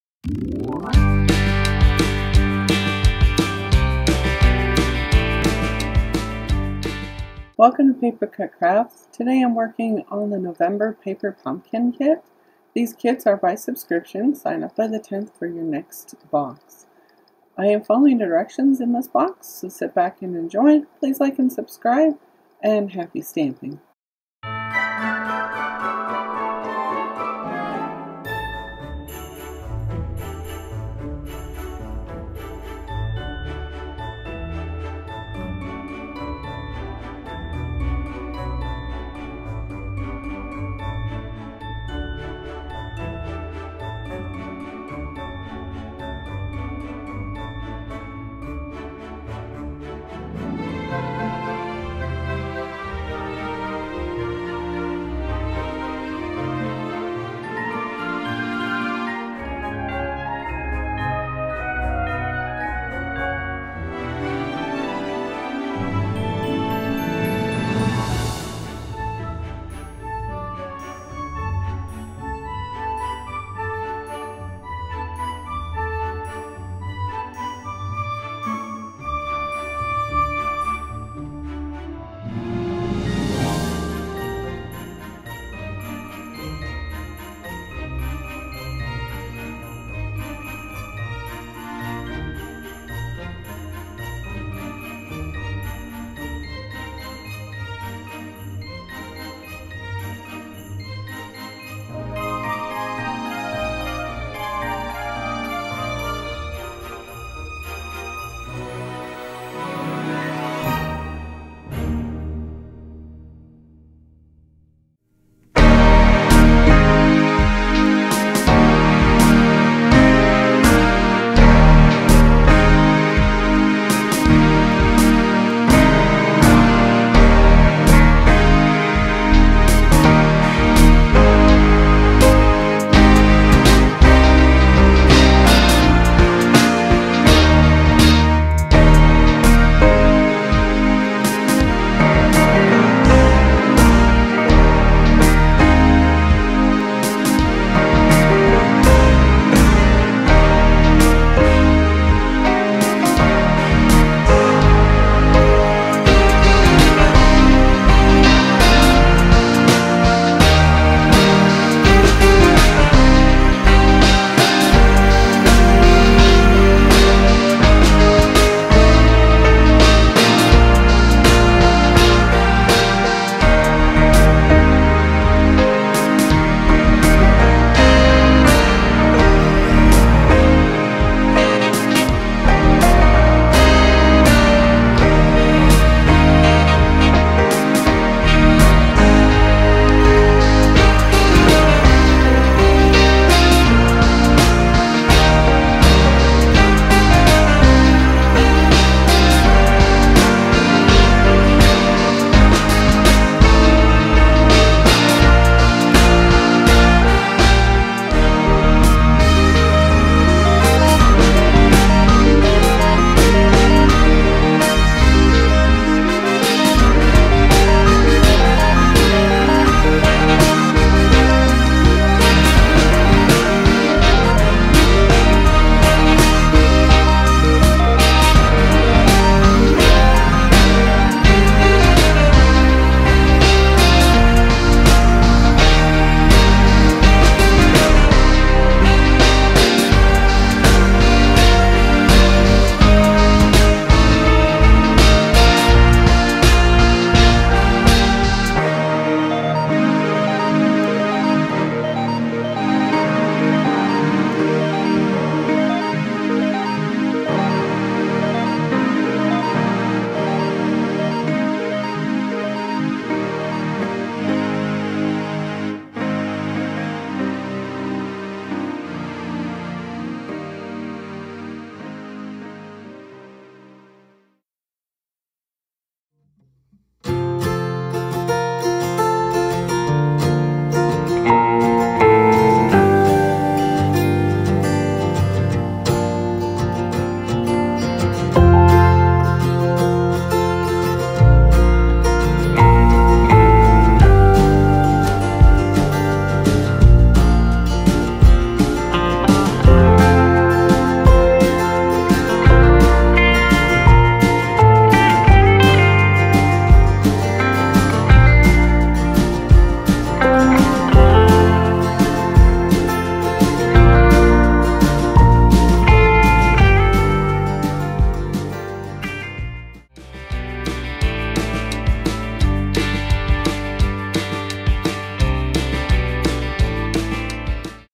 Stampin'Up!® Paper Pumpkin® Put Together
I'm putting together the November Paper Pumpkin® kit from Stampin'Up!®. It's a bunch of cute little gingerbread house tags, note cards and gift card envelopes.  Want a box of your own?